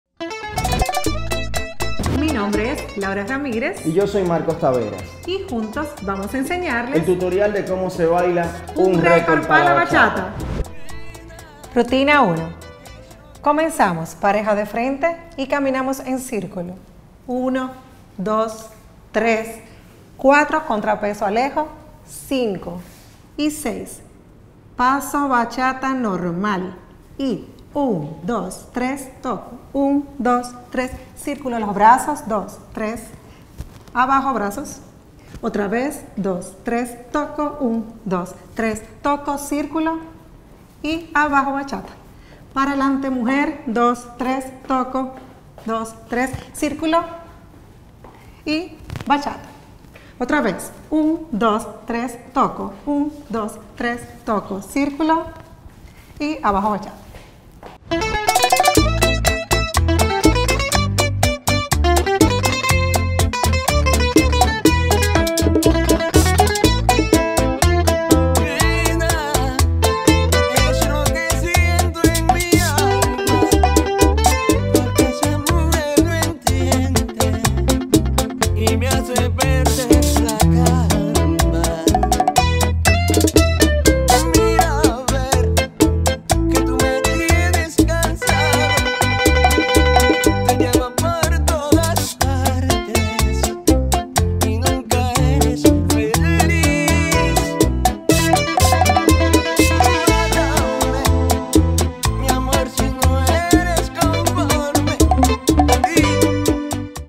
Un Record Pa La Bachata - RUTINA 1
Bailaremos BACHATA por la DOMINICANIDAD
11 DICIEMBRE 2021 Malecón Santo Domingo